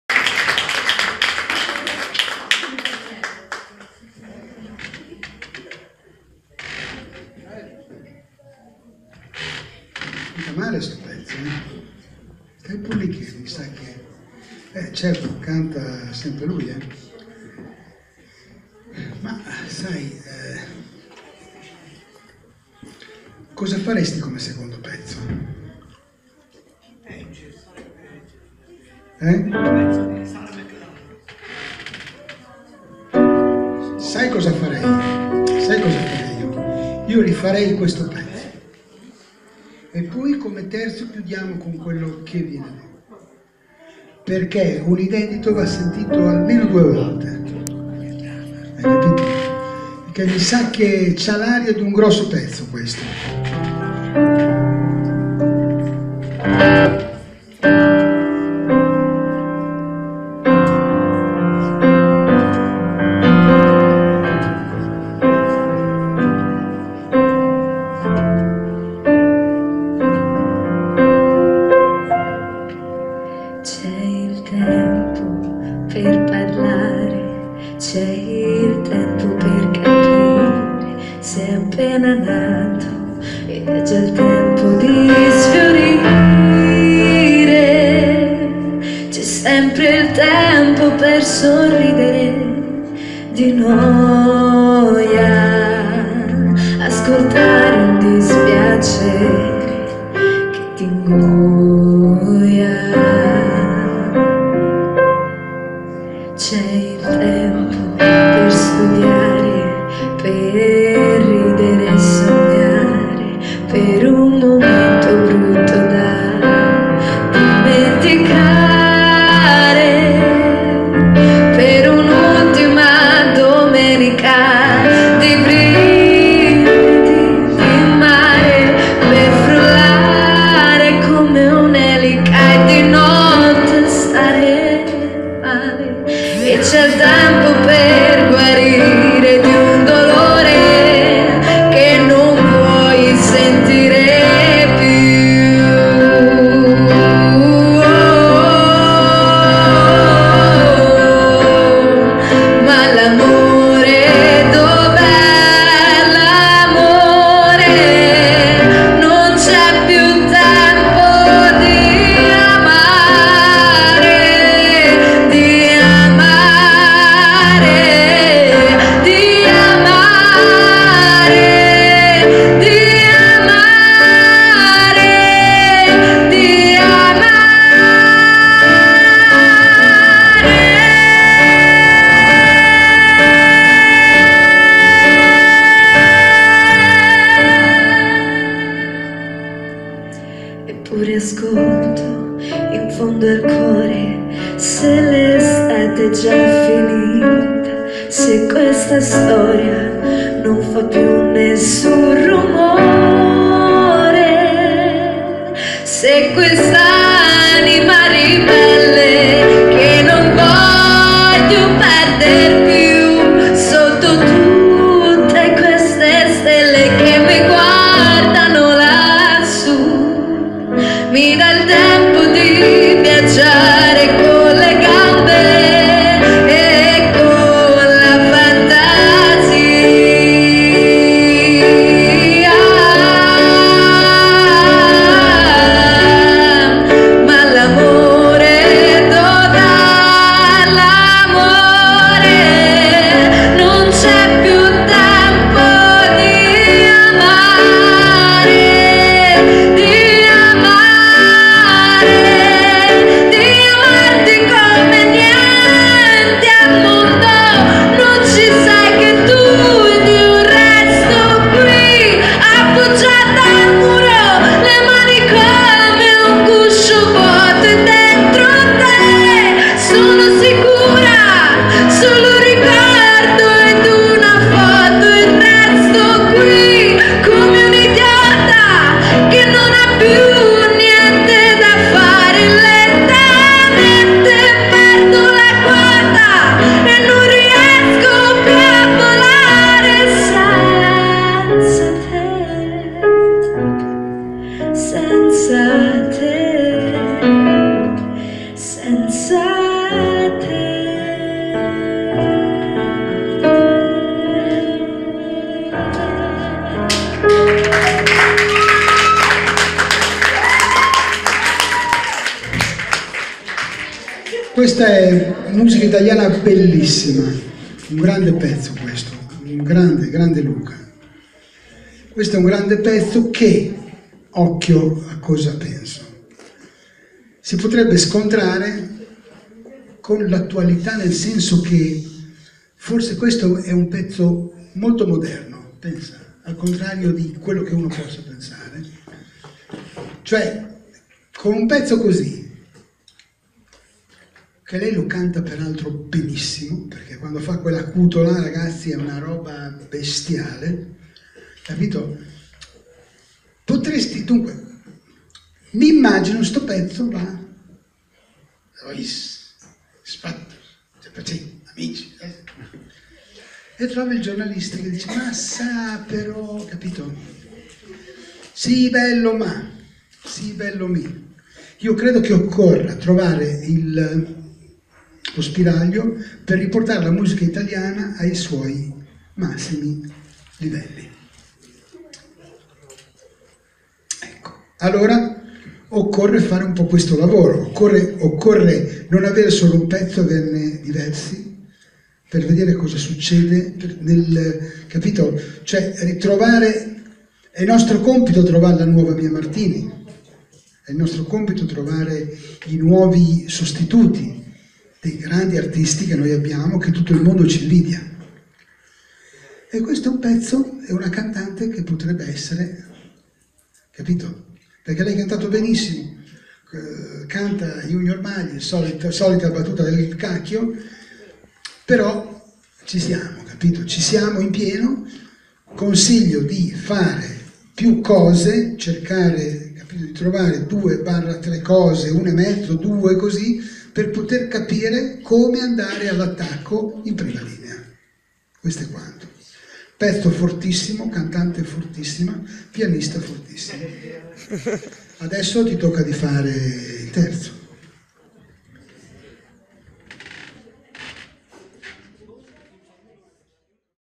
Luca Bechelli, piano con Fio Zanotti e Giulia Fredianelli, in un inedito.
Luca, Fio e Giulia all'accademia a Piombino.